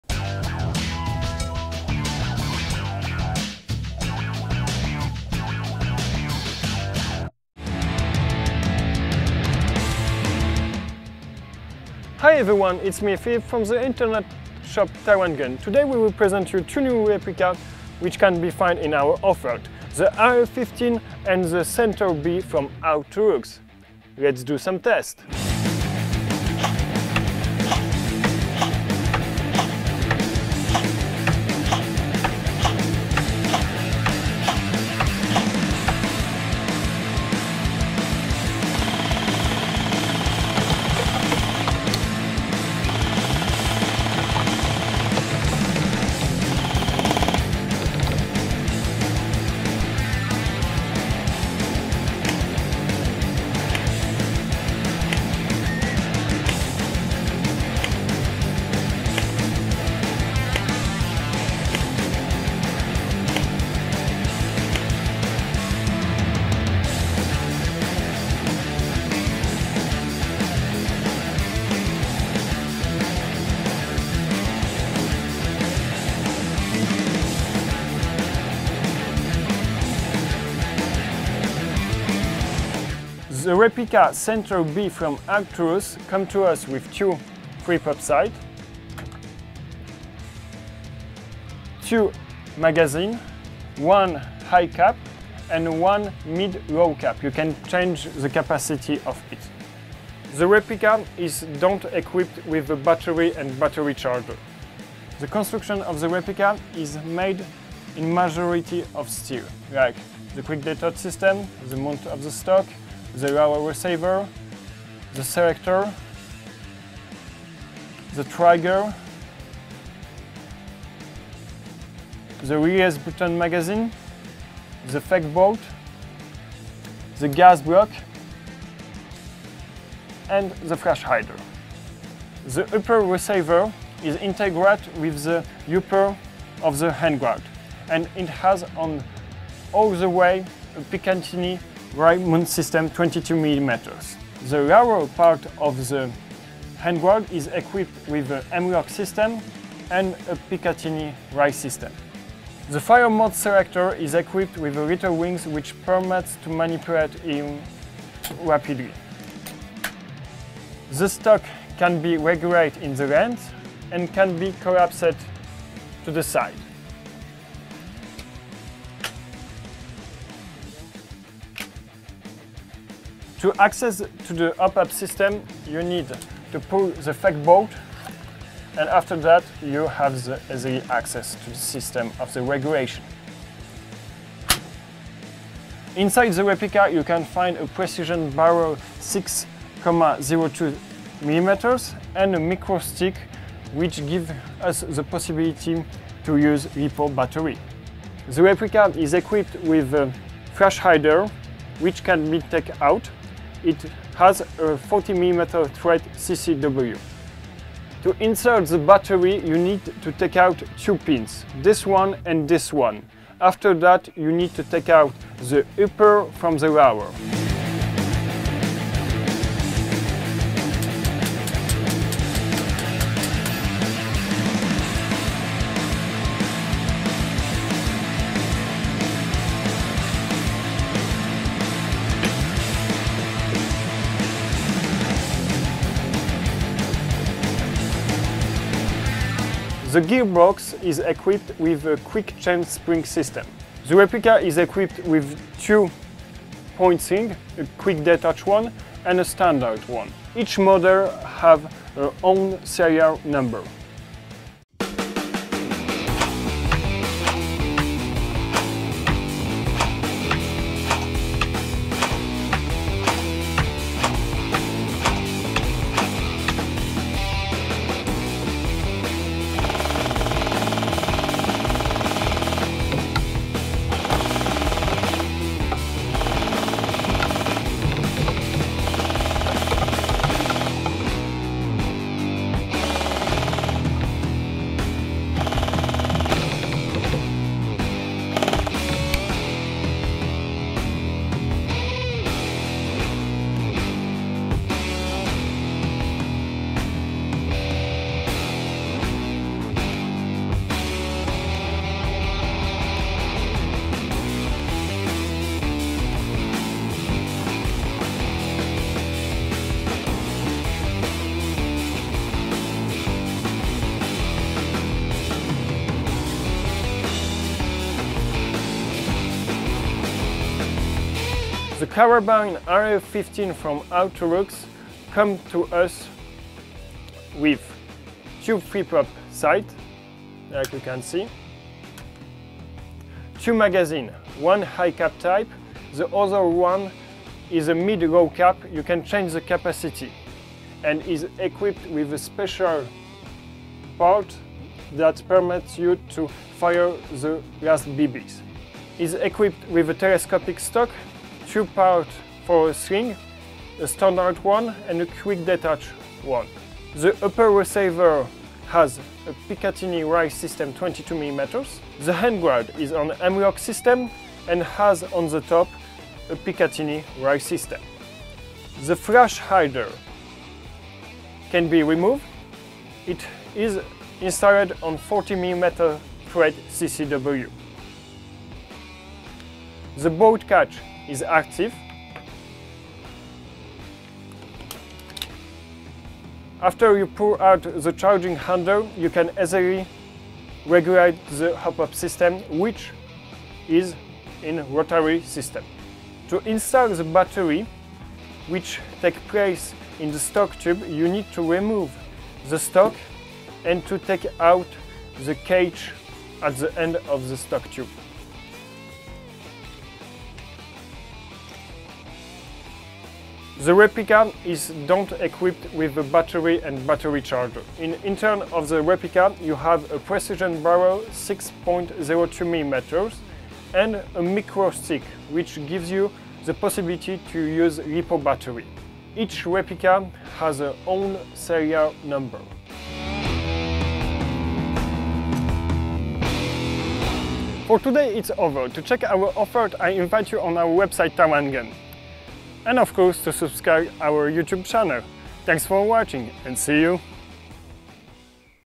Arcturus AR 15/Centaur B - Airsoft Replica Test
Taiwangun ARCTURUS AR 15/Centaur B in Action

Centaur B Technical data:
Weight: 3670 g
Color: black
Length: 722/960 mm
Material: aluminum, steel
Fire mode: auto, single
Length of internal barrel: 430 mm
Diameter of Internal barrel: 6.02 mm
Magazine capacity: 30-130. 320 BBs
Magazine material: polymer
Muzzle velocity: 400 fps
Muzzle energy: 1.49 J
Accumulator included in set: NO
Gearbox version: 9 mm
Warranty period: 12 months
Blowback: no
Number of magazines in set: 2 pcs.
Ammo: .25g, .23g
Manufacturer: Arcturus

Centaur AR 15 Carabine Technical Data:
Weight: 2550 g
Color: black
Length: 890/970 mm
Material: aluminum, steel
Magazine material: metal
Gearbox version: v.2
Warranty period: 12
Blowback: no
Ammo: .25g, .23g
Manufacturer: Arcturus
Code: AT-AR01